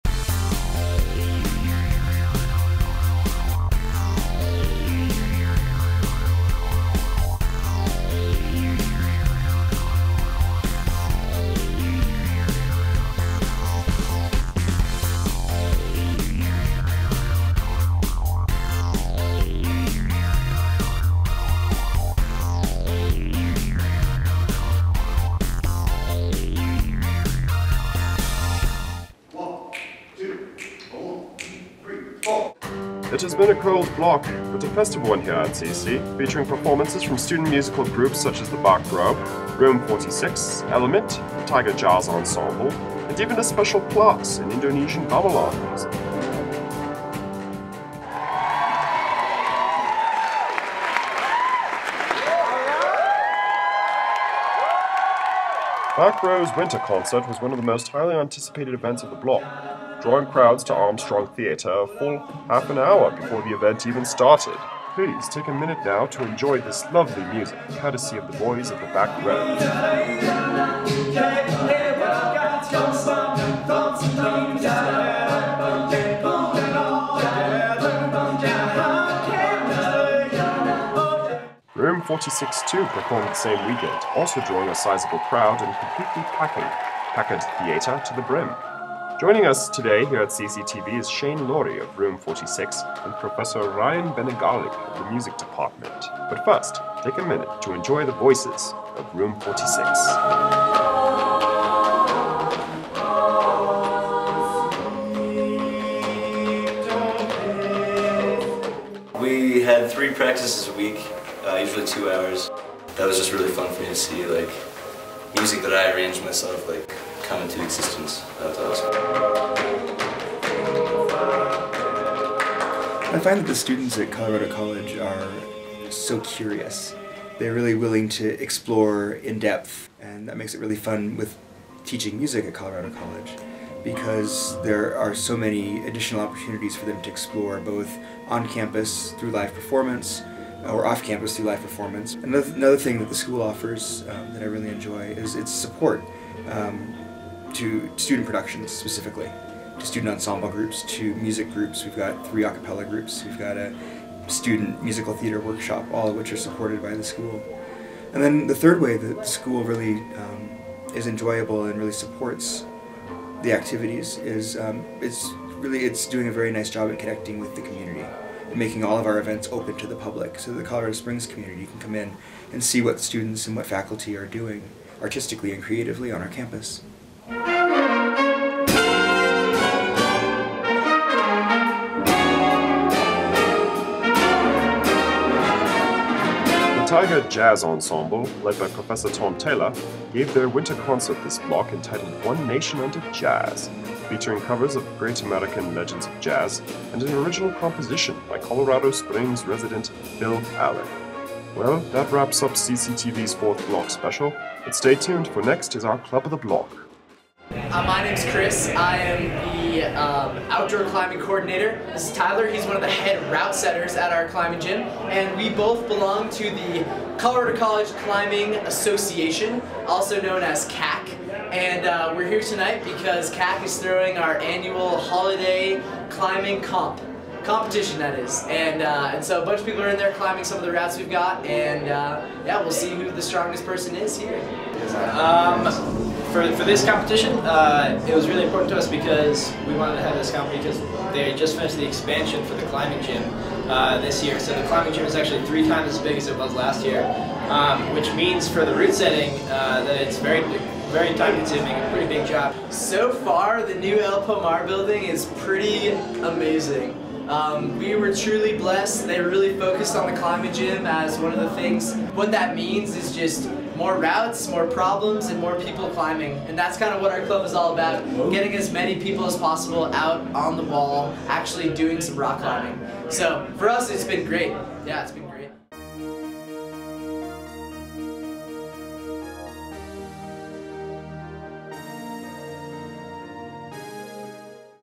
Block 4: Music, Rock Climbing, and more!
The semester has ended, and what a festive 4th Block we've had at Colorado College! This video celebrates music at CC, from our a cappella groups to the Tiger Jazz Ensemble to a very interesting and unique class on Gamelan music! Also features Colorado College Climbing Association as this block's Club Of The Block.